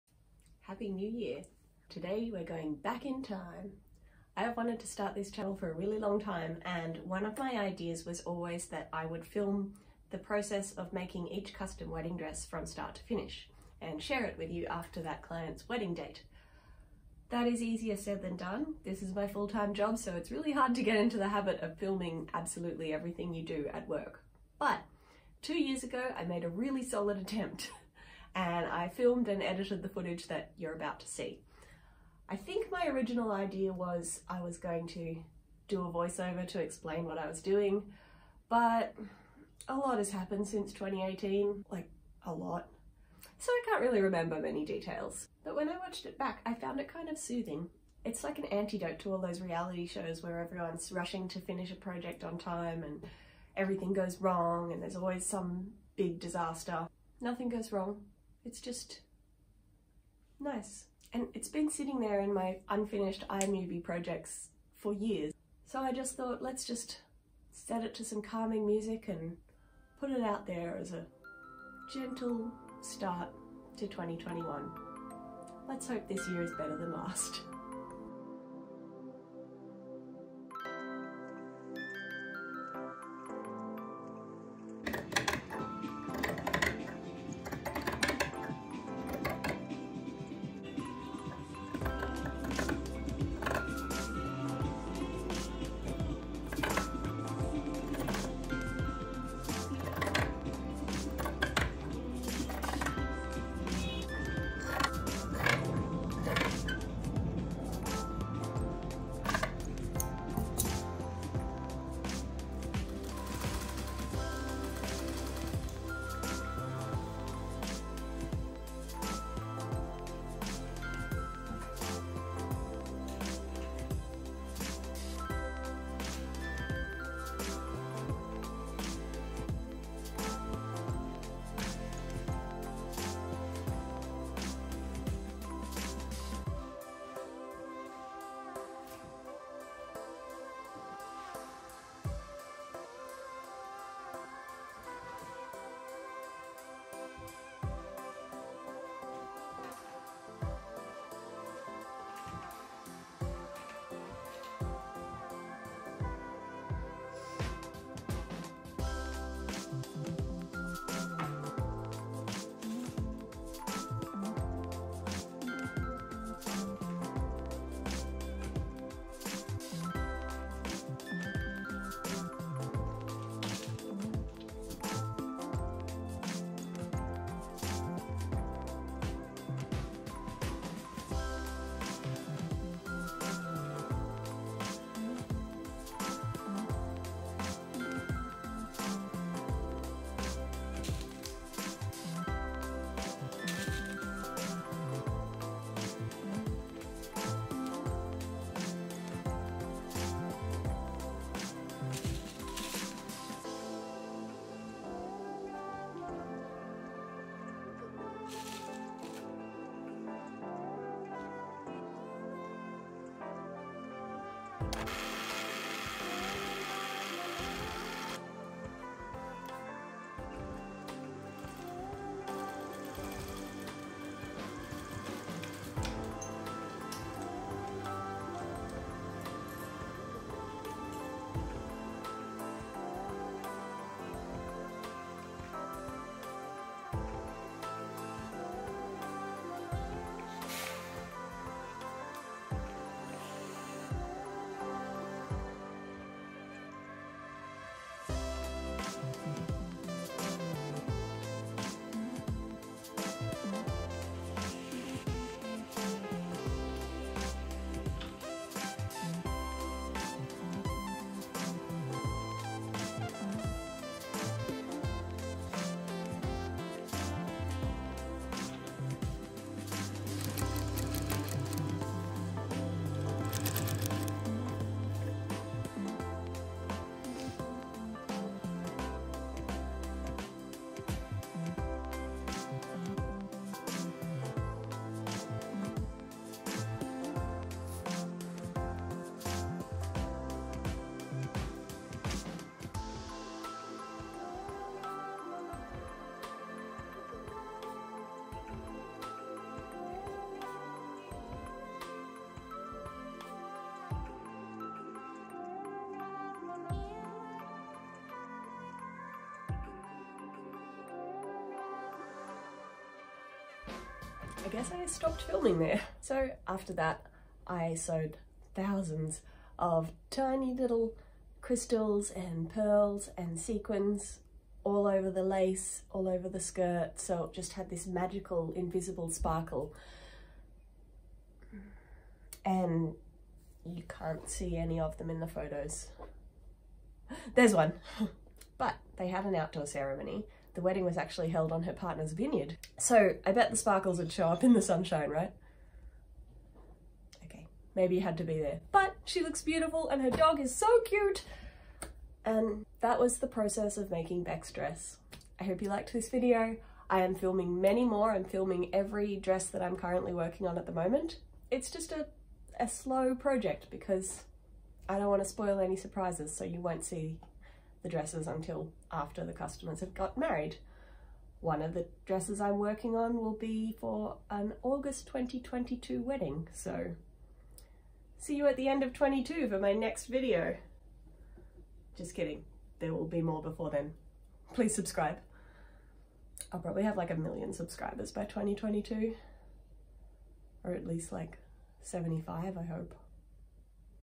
Calm sewing - working on a custom wedding dress
This footage is from an unfinished video I shot in 2018, following the process of making a custom wedding dress.